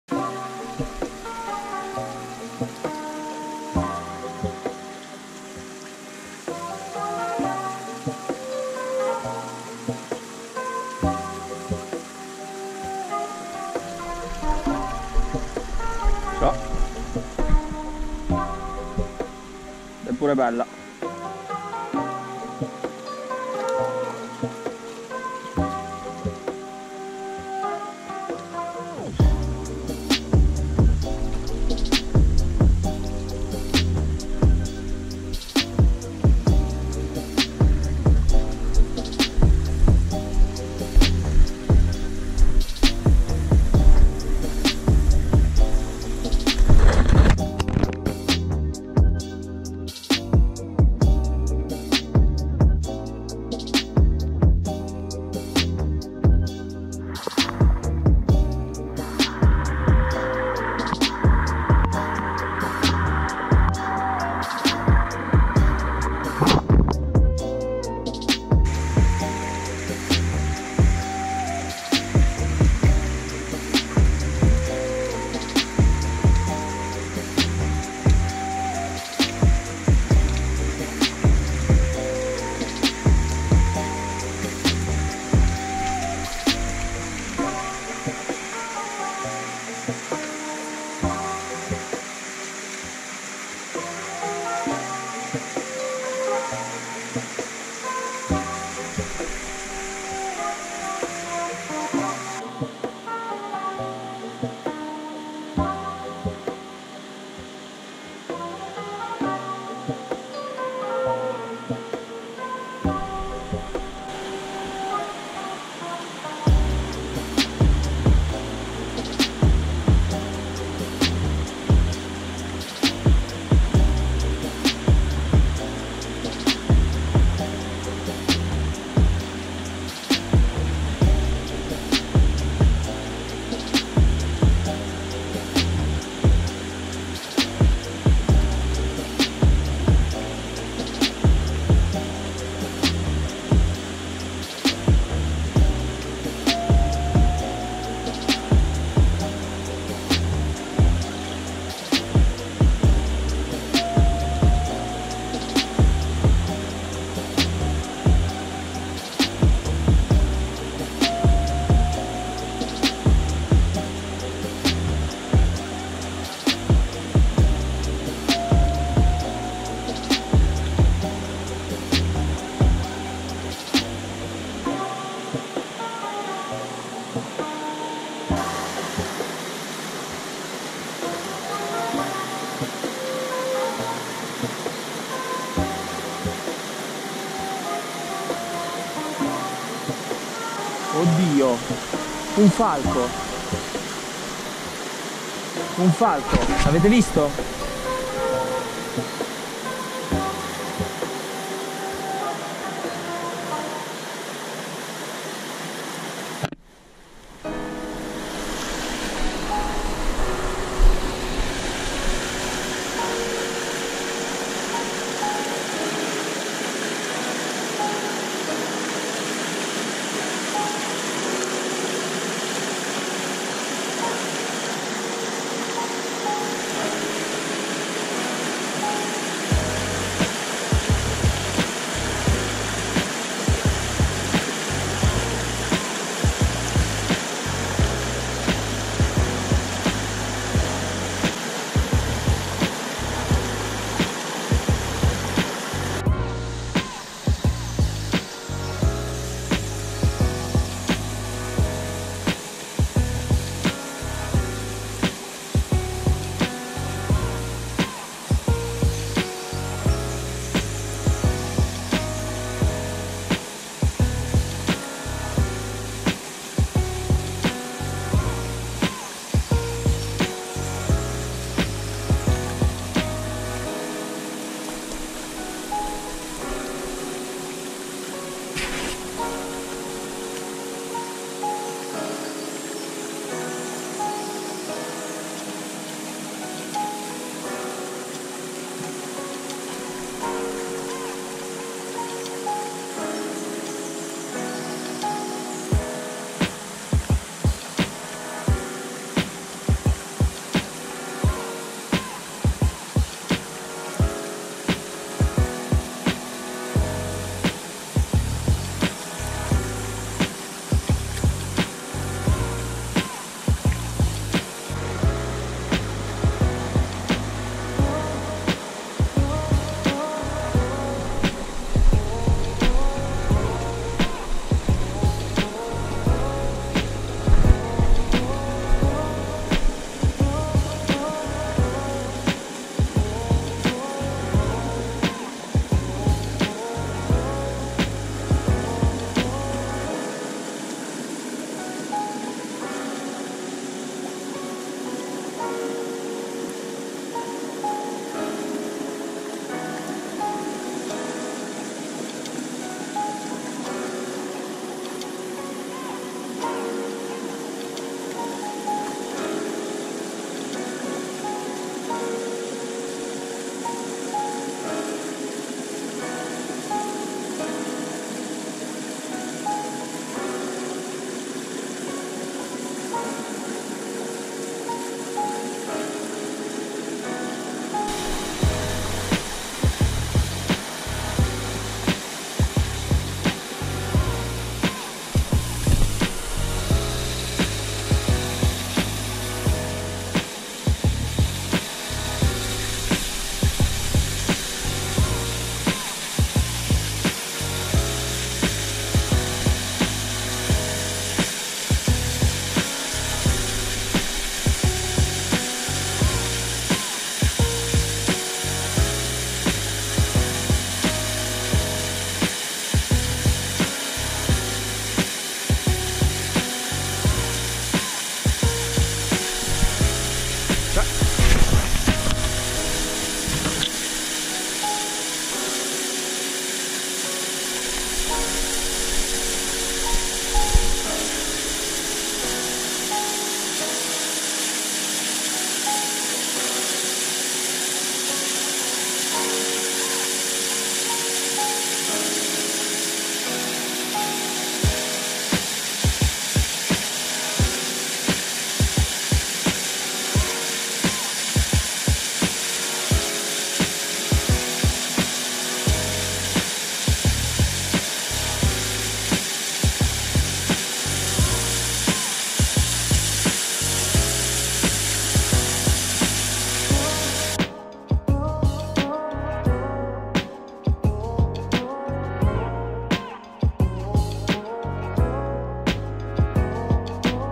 PESCA SPINNING TROTA IN TORRENTE DI MONTAGNA | Una TROTA dopo l'altra
E' successo di tutto in questa sessione di pesca, molte catture, qualcuna anche di taglia interessante, caduto a bagno ed incontro ravvicinato con un falco enorme, anche se dal video non rende non rende molto la taglia...
Commenti e suggerimenti sono ben accetti!!!